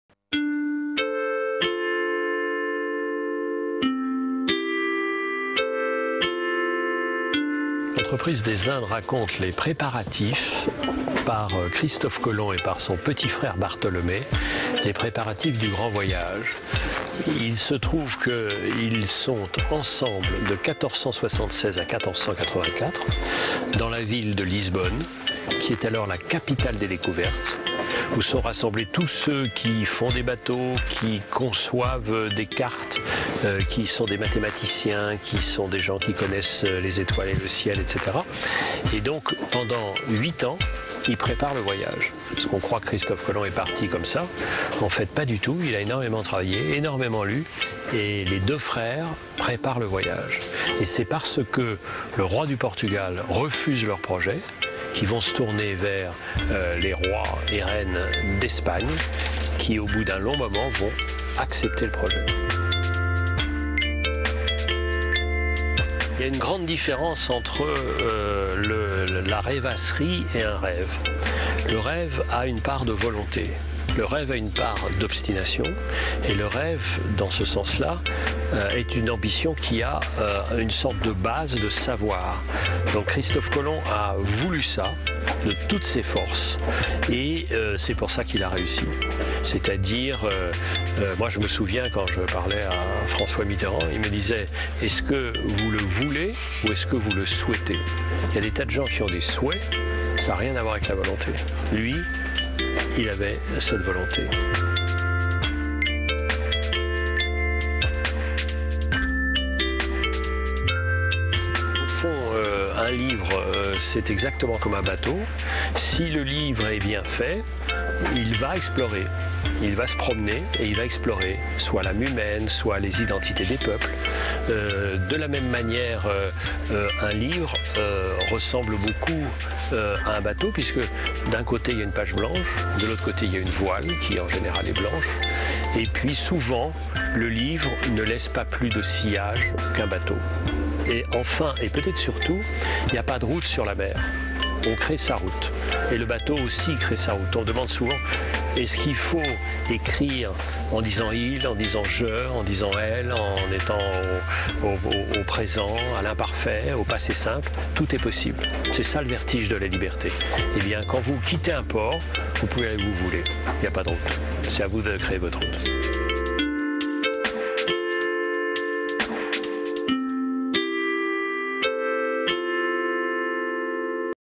Erik Orsenna : "L'entreprise des Indes"
L'écrivain Erik Orsenna aime la Mer et La Littérature, et l'on retrouve toutes ces composantes dans son nouvel ouvrage intitulé "L'entreprise des Indes". (DROITS RESERVES . Pour toute exploitation sur TV, web, mobile, aller